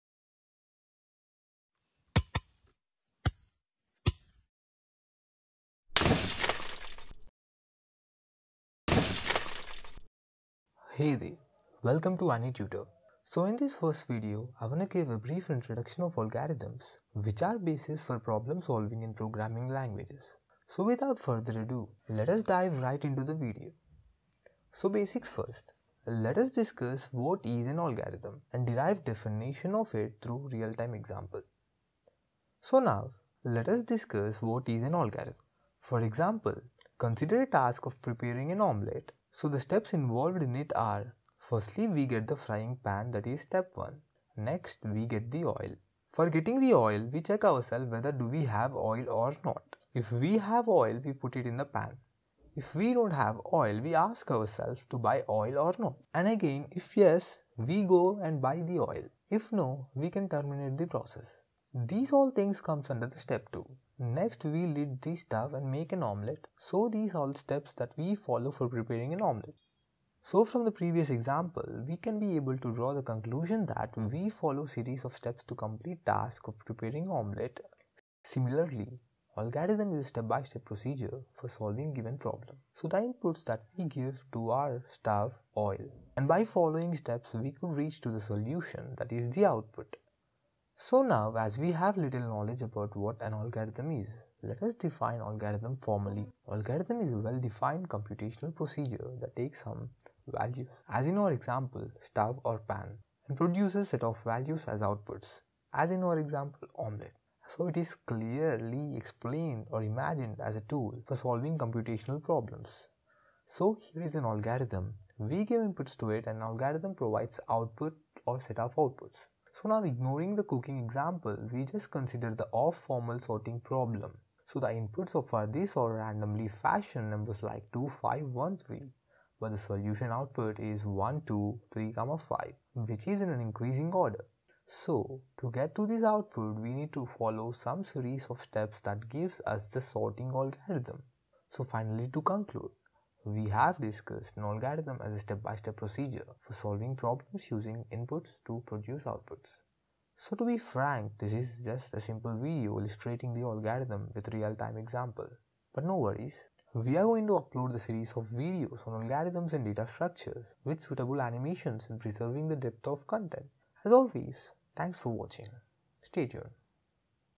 What is an Algorithm ? | Algorithms
Algorithms and formal definition of it with real time example.

This video is intro about algorithms , in the series Algorithms